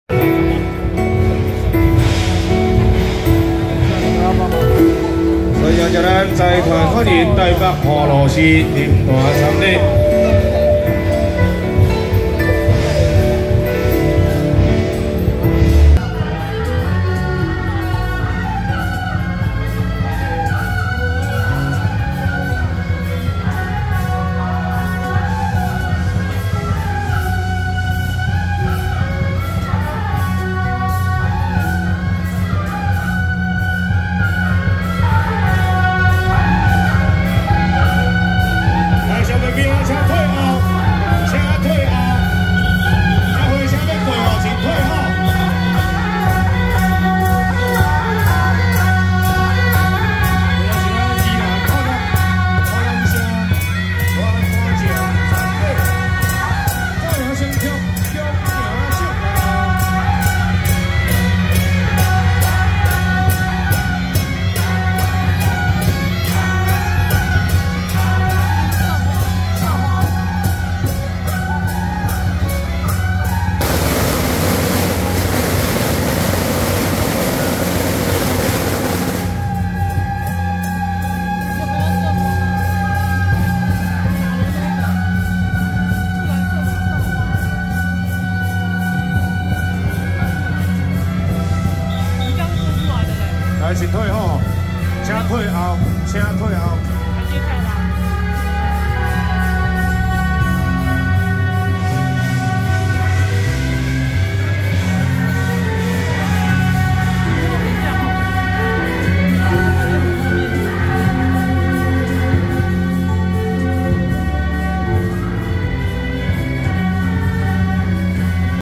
【宜蘭復蘭社-鮮花車大鼓弄】~113騏瑋集團關聖帝君謁祖進香回鑾暨騏瑋集團安龍謝土慶成三週年慶典遶境
宜蘭復蘭社-鮮花車大鼓弄